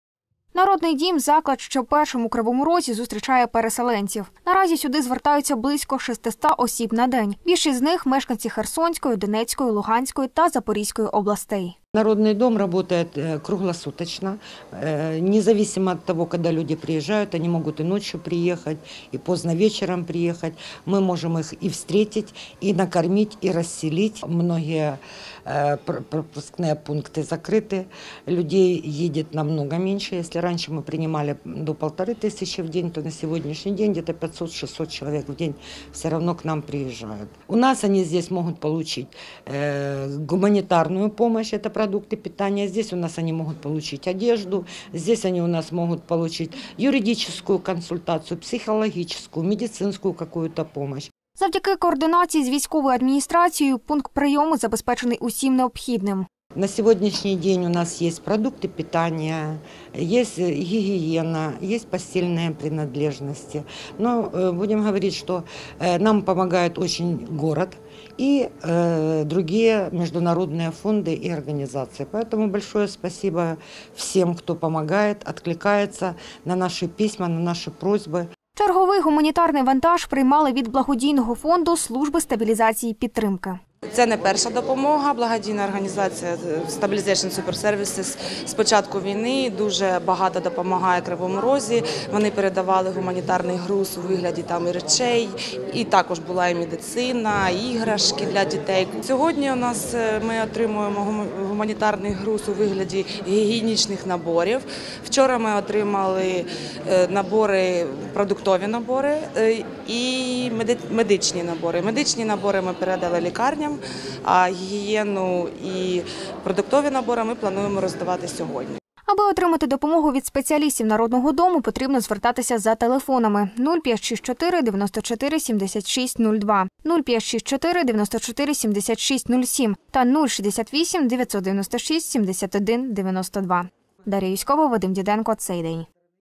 Зустрічають переселенців: у народному домі - понад пів тисячі відвідувачів щодня
Більш як півтисячі переселенців щодня. І вдень, і вночі. В такому режимі працює зараз Народний дім. Люди отримують продукти, одяг, мають змогу звернутися по допомогу до юристів, медиків, психологів. Дар’я Юзькова побувала у пункті прийму переселенців.